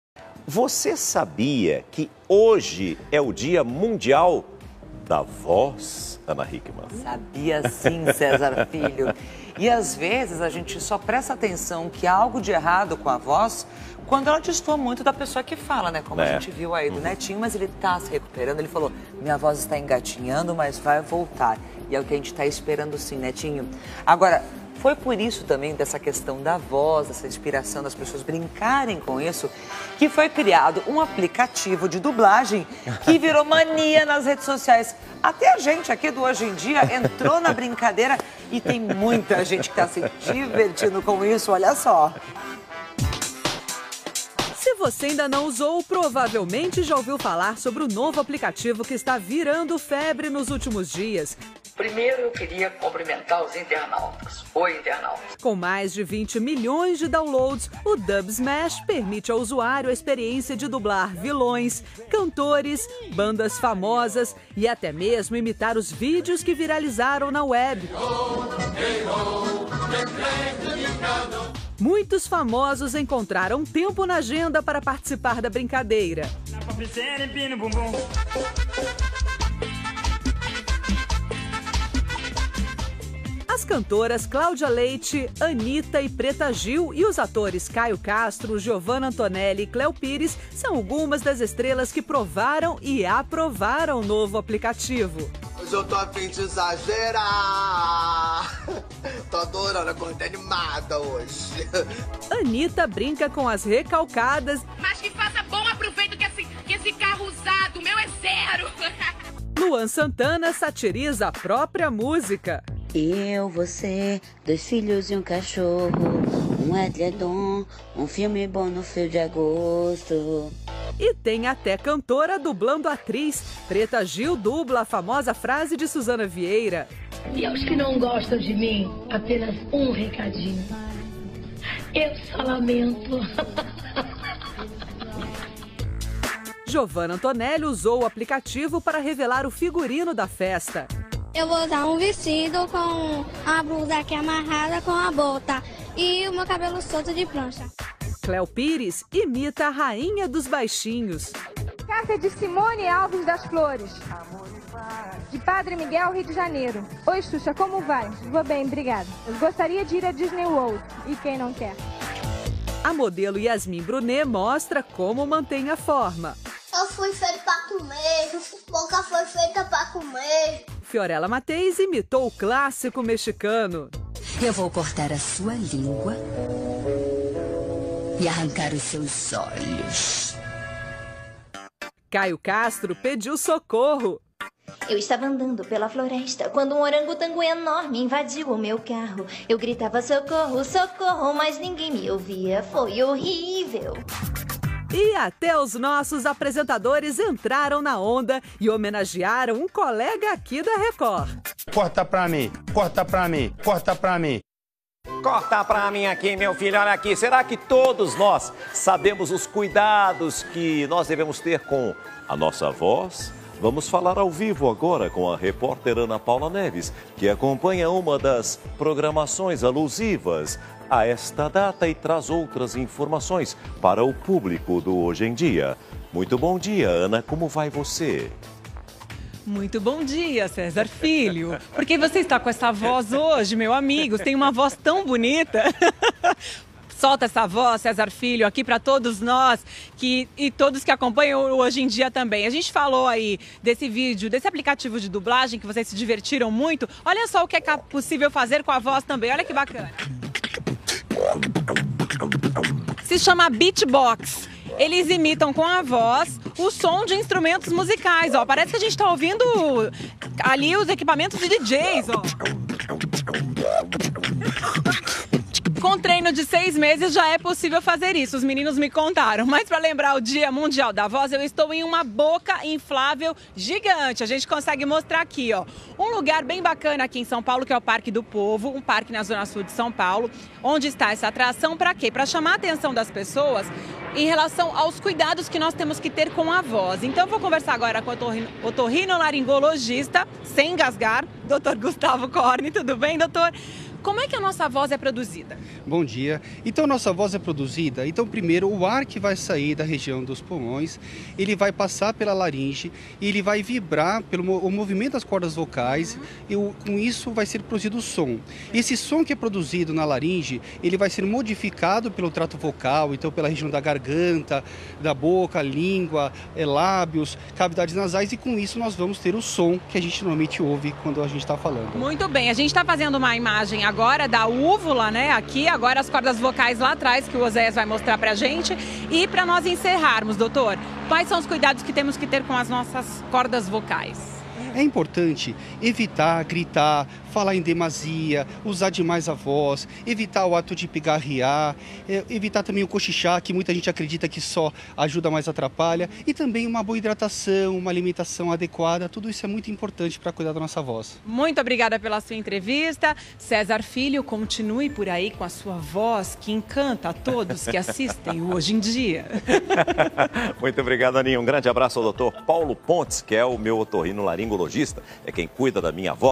No Dia Mundial da Voz, especialista mostra como cuidar das cordas vocais
Nesta quinta-feira (16) é comemorado o Dia Mundial da Voz e para que ela esteja sempre em perfeito estado é importante tomar alguns cuidados. O otorrinolaringologista Gustavo Korn passou algumas dicas preciosas. Veja!